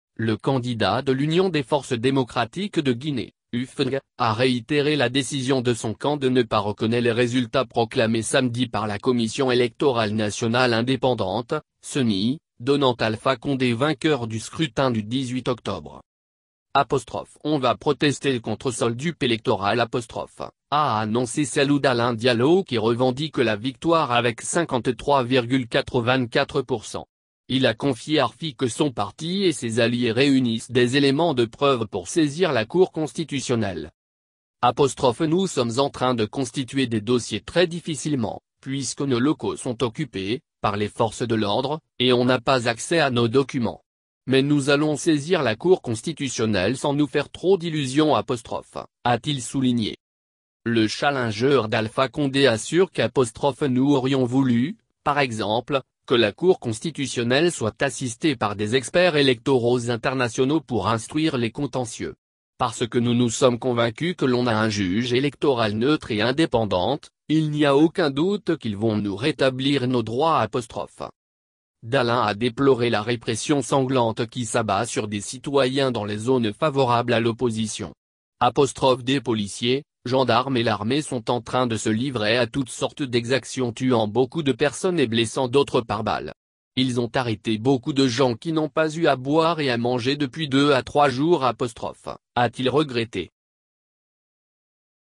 Gestion des contentieux électoraux : Dalein souhaite que la Cour constitutionnelle soit assistée p
Le candidat de l’Union des forces démocratiques de Guinée (UFDG) a réitéré la décision de son camp de ne pas reconnait les résultats proclamé samedi par la Commission électorale nationale indépendante (CENI), donnant Alpha Condé vainqueur du scrutin du 18 octobre.

‘’On va protester contre ce holdup électoral’’, a annoncé Cellou Dalein Diallo qui revendique la victoire avec 53,84%. Il a confié à RFI que son parti et ses alliés réunissent des éléments de preuves pour saisir la Cour constitutionnelle.

‘’Nous sommes en train de constituer des dossiers très difficilement, puisque nos locaux sont occupés [par les forces de l’ordre] et on n’a pas accès à nos documents. Mais nous allons saisir la Cour constitutionnelle sans nous faire trop d’illusions’’, a-t-il souligné.

Le challenger d’Alpha Condé assure que ‘’nous aurions voulu, par exemple, que la Cour constitutionnelle soit assistée par des experts électoraux internationaux pour instruire les contentieux. Parce que nous nous sommes convaincus que l’on a un juge électoral neutre et indépendante, il n’y a aucun doute qu’ils vont nous rétablir nos droits’’.

Dalein a déploré la répression sanglante qui s’abat sur des citoyens dans les zones favorables à l’opposition. ‘’Des policiers, gendarmes et l’armée sont en train de se livrer à toutes sortes d’exactions tuant beaucoup de personnes et blessant d’autres par balles. Ils ont arrêté beaucoup de gens qui n’ont pas eu à boire et à manger depuis deux à trois jours’’, a-t-il regretté.